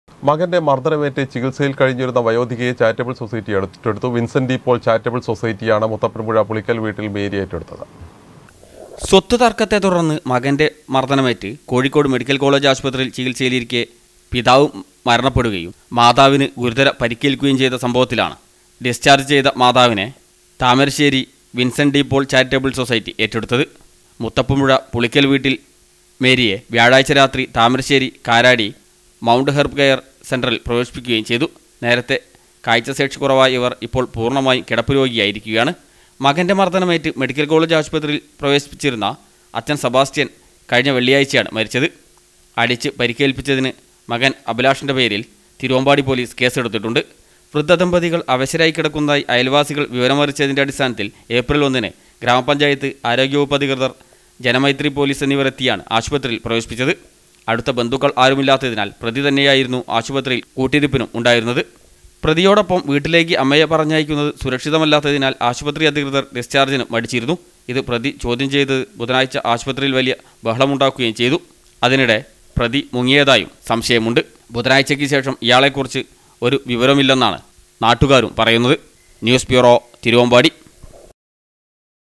മകൻ്റെ മർദനമേറ്റ് ചികിത്സയിൽ കഴിഞ്ഞിരുന്ന വയോധികയെ ചാരിറ്റബ്ൾ സൊസൈറ്റി ഏറ്റെടുത്തു
മകൻ്റെ മർദനമേറ്റ് ചികിത്സയിൽ കഴിഞ്ഞിരുന്ന വയോധികയെ ചാരിറ്റബ്ൾ സൊസൈറ്റി ഏറ്റെടുത്തു. വിൻസൻ്റ് ഡി പോൾ ചാരിറ്റബ്ൾ സൊസൈറ്റിയാണ് മുത്തപ്പൻ പുഴപുളിക്കൽ വീട്ടിൽ മേരിയെ ഏറ്റെടുത്തത്